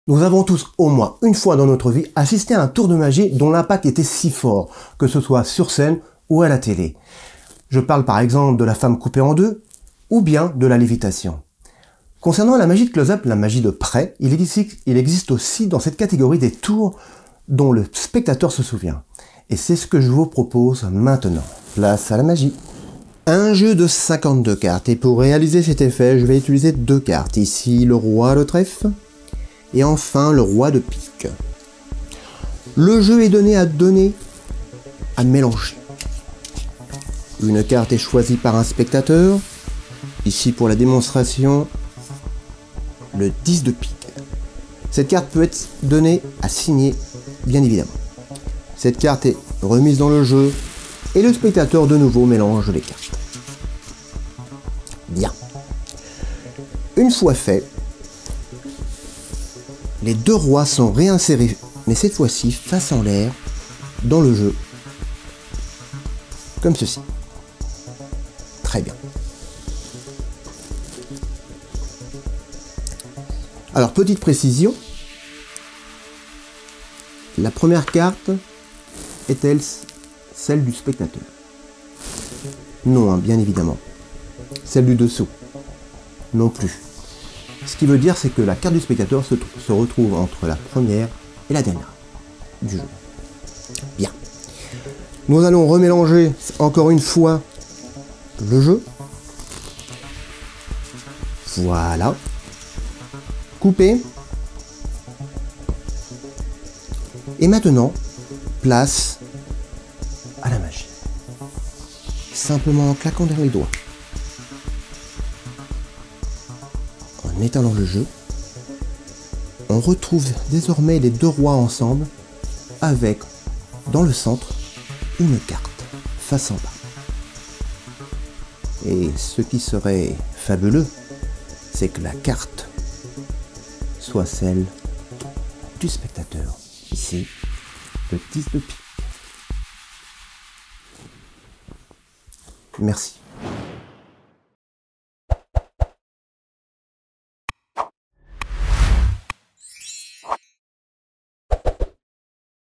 TOUR DE MAGIE SANDWICH REVOLUTION 🌟🌟🌟
Un tour de magie avec des cartes du petit magicien réalisé par Magic Pascal
Les routines de cartes avec les effets sandwich sont nombreuses, mais celui là est exceptionnel!

Magic Pascal